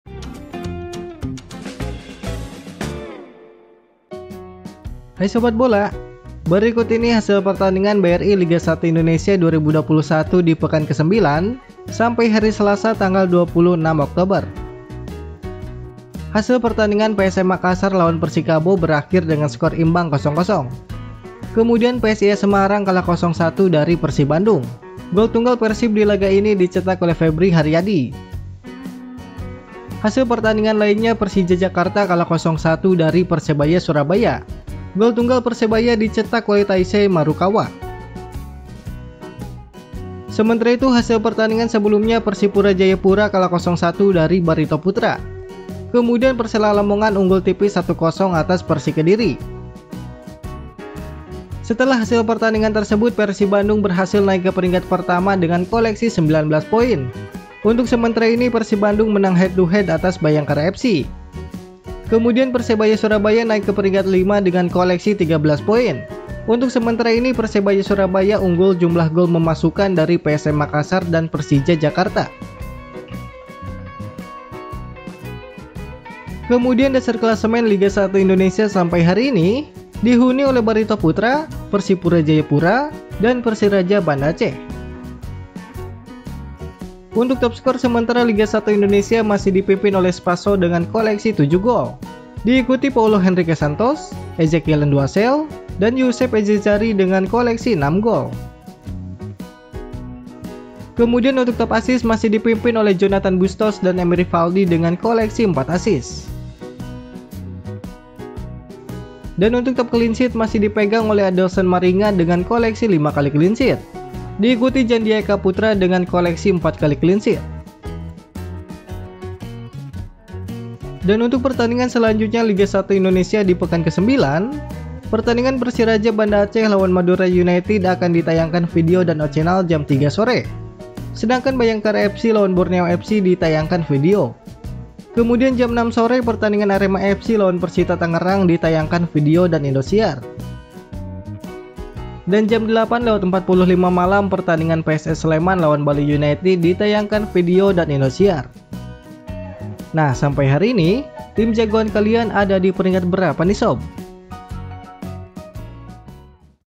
Hasil & Klasemen BRI Liga 1 Terbaru - Persija vs Persebaya, PSIS vs Persib Bandung
Update klasemen BRI Liga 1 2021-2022 Selasa 26 Oktober 2021, setelah hasil pertandingan Liga 1 terbaru Persija Jakarta vs Persebaya Surabaya 0-1, PSIS Semarang vs Persib Bandung 0-1, Top Skor dan Jadwal Liga 1 hari ini Live Indosiar dan streaming vidio lengkap Pekan 9
Full time PSM Makassar vs Persikabo 0-0
Full time PSIS Semarang vs Persib Bandung 0-1
Full time Persija Jakarta vs Persebaya Surabaya0-1
Full time Persipura Jayapura 0-1 PS Barito Putera
Full time Persela Lamongan 1-0 Persik Kediri

Jadwal Selanjutnya Liga 1 hari ini
15:15 WIB : Persiraja Banda Aceh vs Madura United Live Vidio, O Channel
15:15 WIB : Bhayangkara FC vs Borneo FC Live Vidio
18:15 WIB : Arema FC vs Persita Tangerang Live Vidio, Indosiar
20:45 WIB : PSS Sleman vs Bali United FC Live Vidio, Indosiar

*Jika ada perubahan jadwal akan segera diupdate